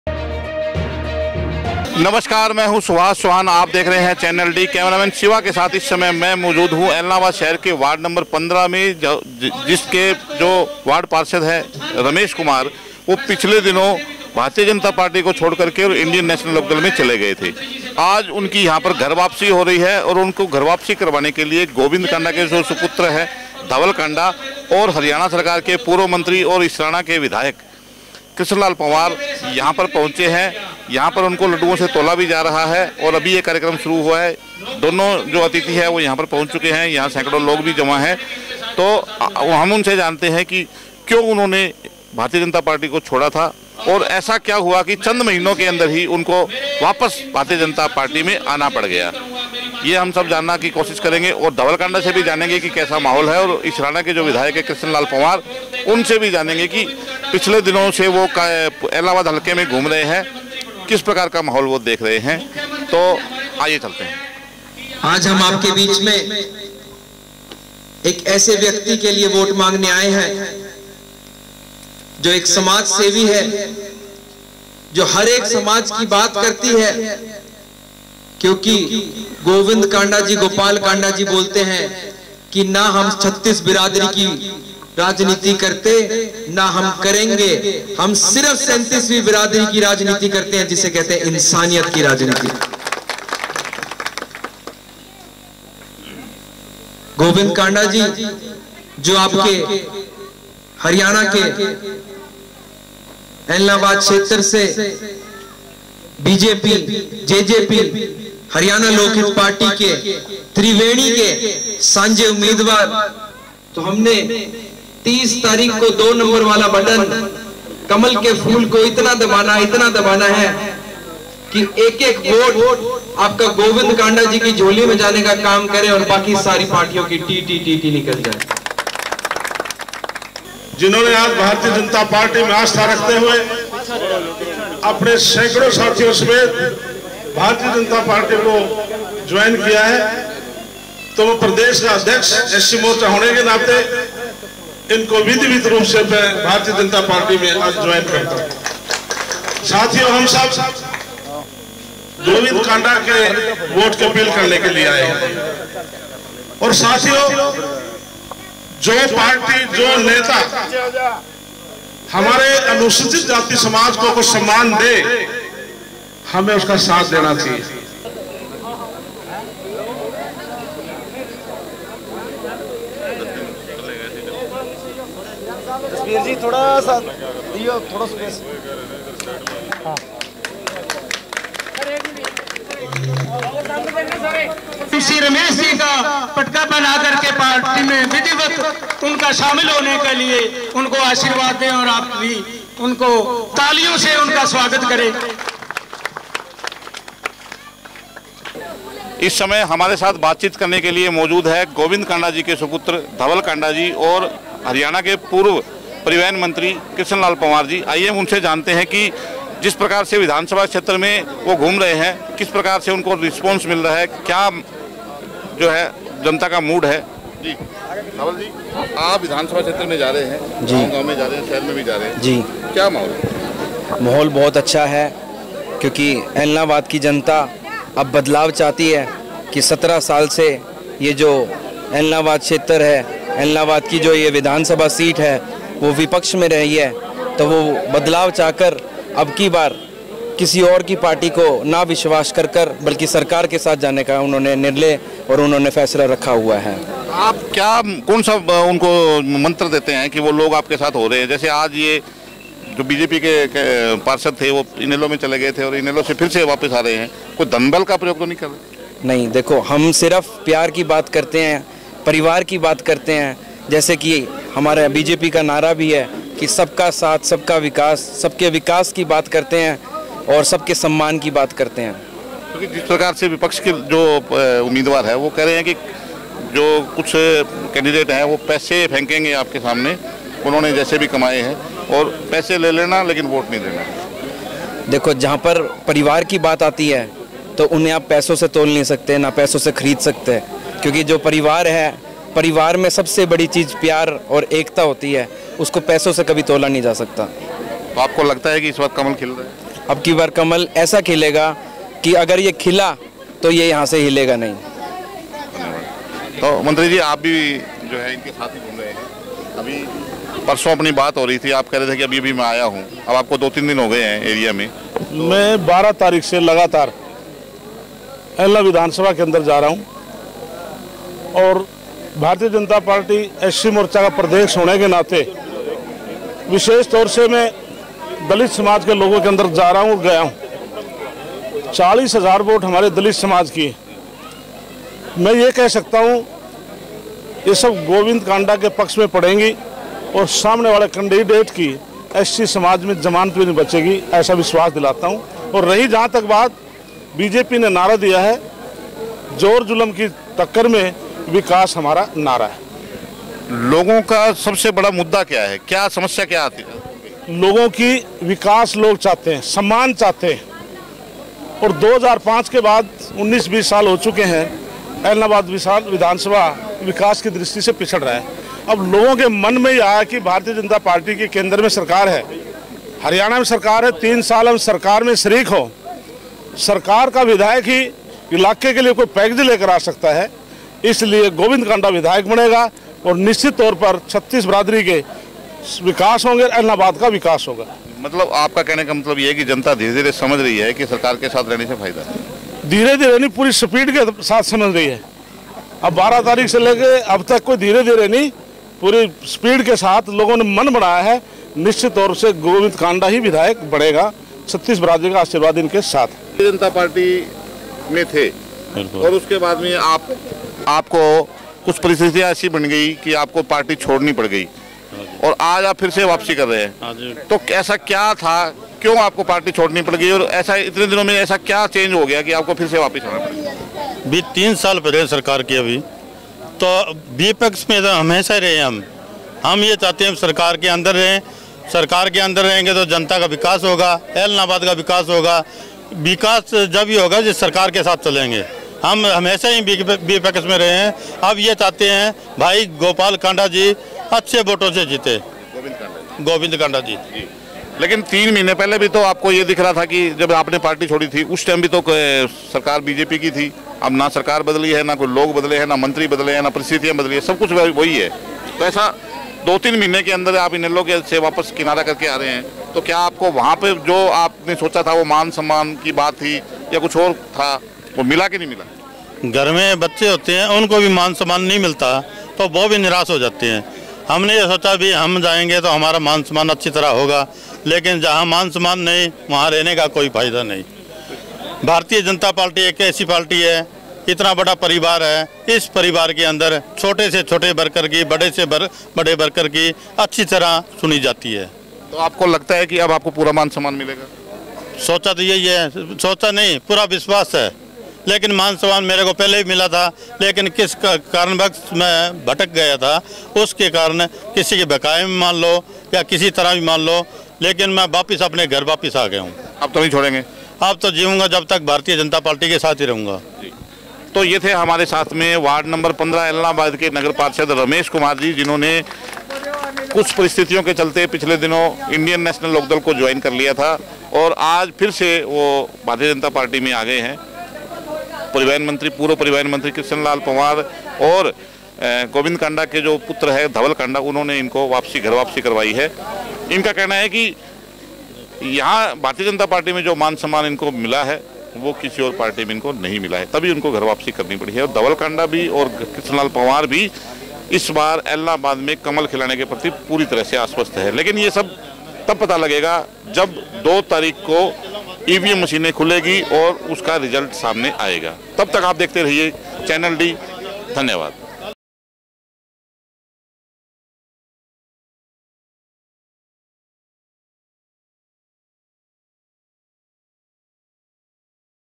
पार्षद रमेश को इनेलो में नही मिला सम्मान कांडा व पंवार ने करवाई घर वापसी | Channel D | ByElection
Report : Subhash Chauhan

Cameraman : Sachin Moyal

Video Editor : Rohit  Chauhan

Studio : Social Squad